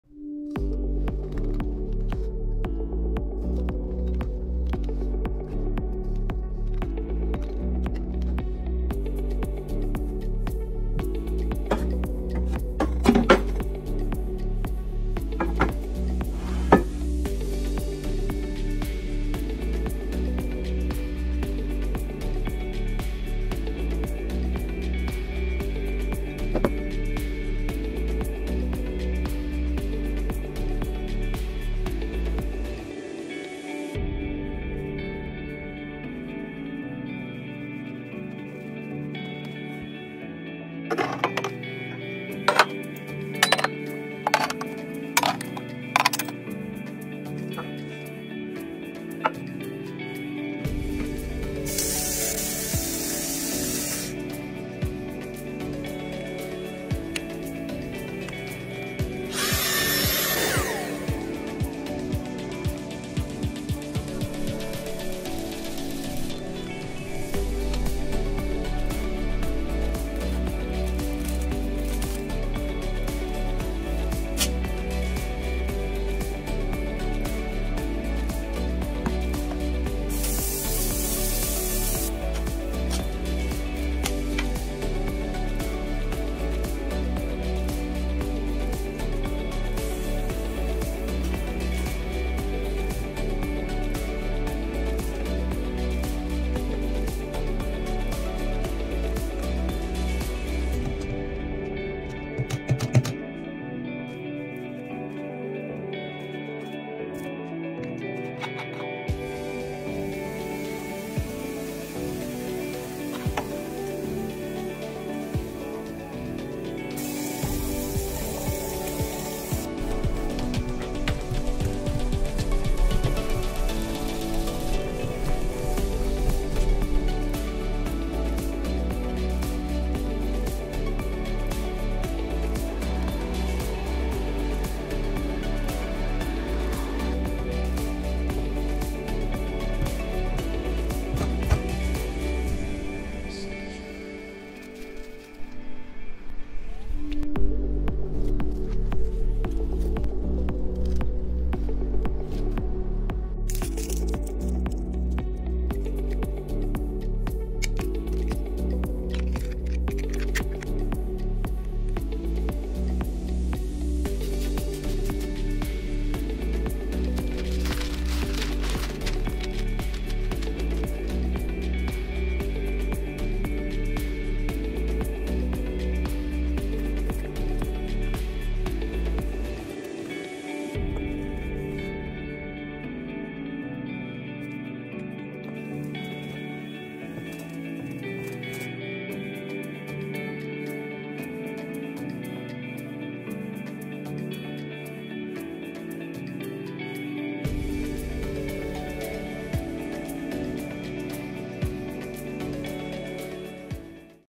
Genius home hacks
Improve your home’s efficiency, organization, and style with the top home hacks I use every day. This original video was produced by Network Media LLC and Liz & Jeff.